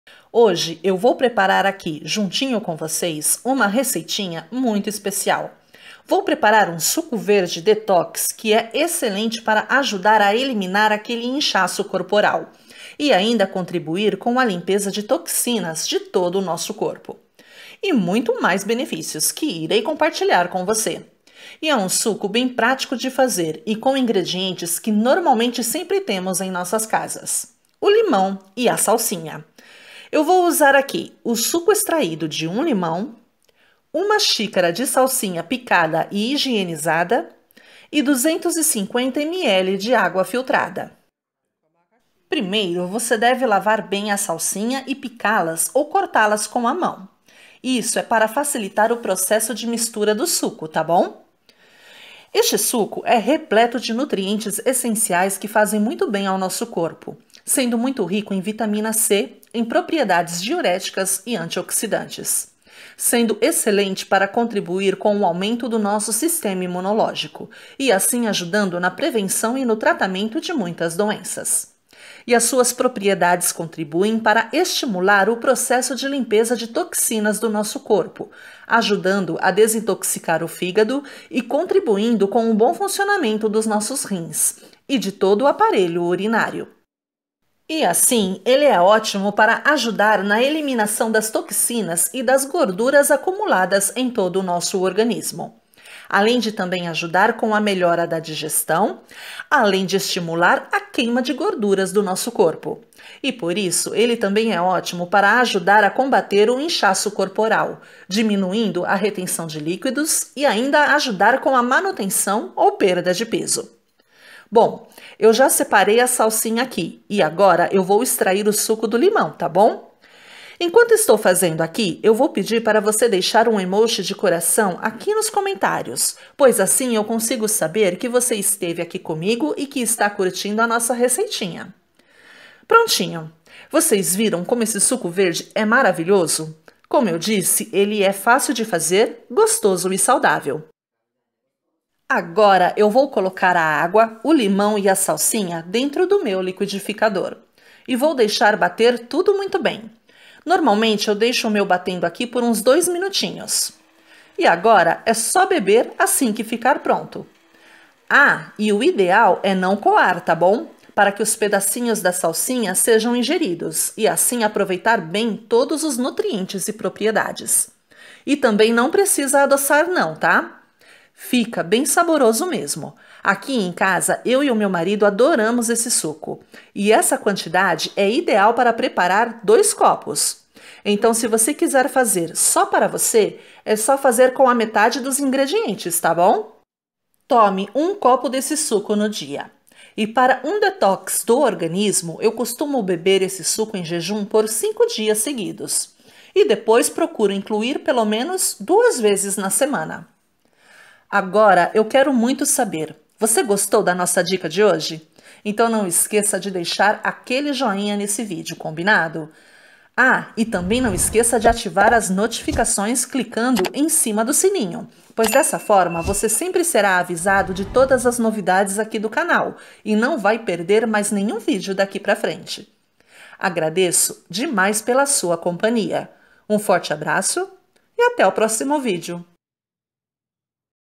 Esta FOLHA Limpa os RINS e FÍGADO Rapidamente, Eliminando a GORDURA DO FÍGADO e Todo o INCHAÇO
Hoje eu vou fazer aqui para vocês uma super receitinha, que além de ser muito refrescante, vai te ajudar bastante a cuidar da saúde dos seus rins, do seu fígado, vai ajudar a combater a retenção de líquidos, acaba com a gordura no fígado, contribuindo para eliminar o excesso de inchaço do nosso corpo e ainda vai ajudar no processo de emagrecimento.
Muito bom não é mesmo?
Então é só prestar bastante atenção em nosso vídeo de hoje.

AVISO: Antes de usar qualquer tratamento alternativo consulte sempre um especialista ou profissionais da área.